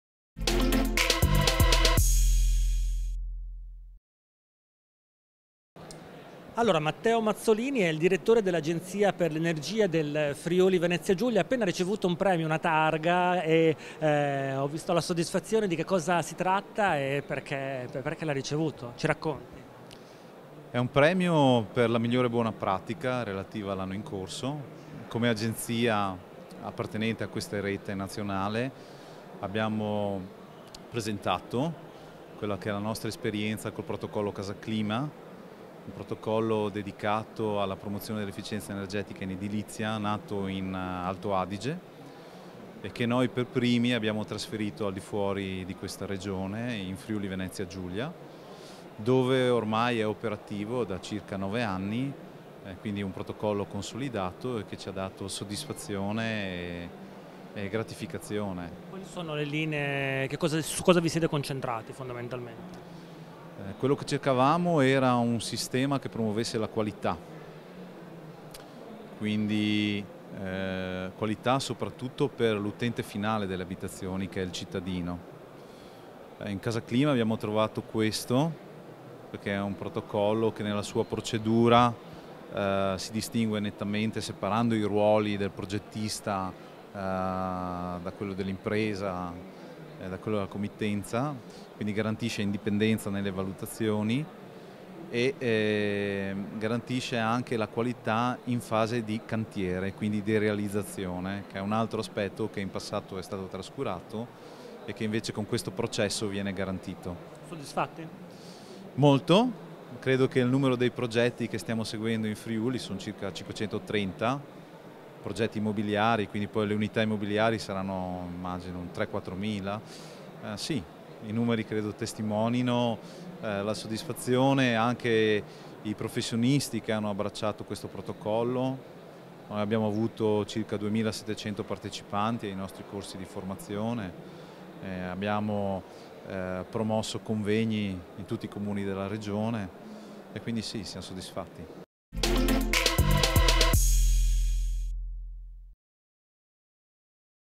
SAIE 2015 - Premio per la migliore buona pratica a Matteo Mazzolini
Matteo Mazzolini, direttore dell’agenzia per l'Energia del Friuli Venezia Giulia, riceve a SAIE 2015 il premio Renael grazie al protocollo “Casa Clima” dedicato alla promozione dell’efficienza energetica in edilizia.
SAIE SMART HOUSE 2015, il grande Salone italiano dell'Edilizia, a BolognaFiere dal 14 al 17 ottobre.
video by Publivideo 2